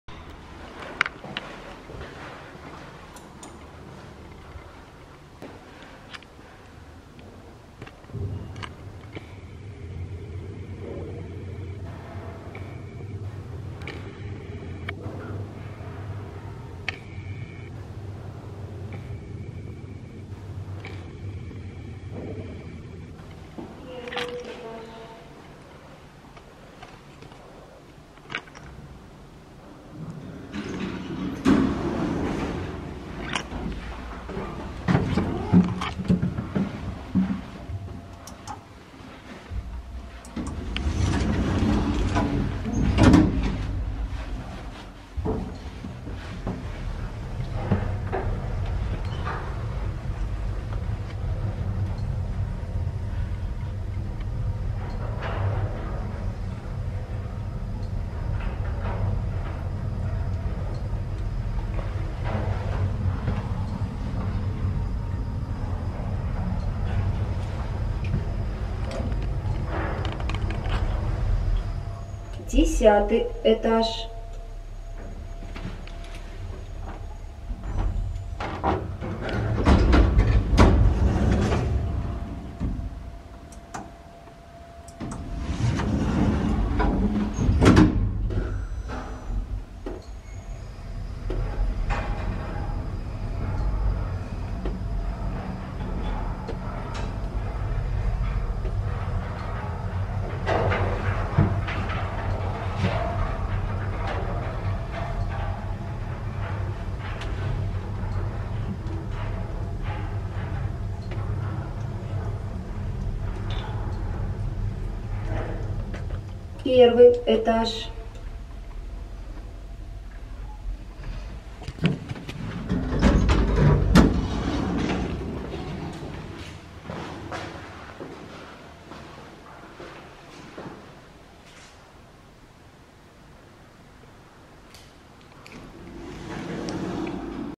Лифт NLM (2020 Г.В) V=1 М/C, Грузоподъемность Q=400 КГ.
Адрес : Нижнекамск, Корабельная улица, 31, Подъезд № 1
Год постройки: 1994
Количество этажей: 10
Жилых помещений: 266
Модель камеры Canon PowerShot SX 540 HS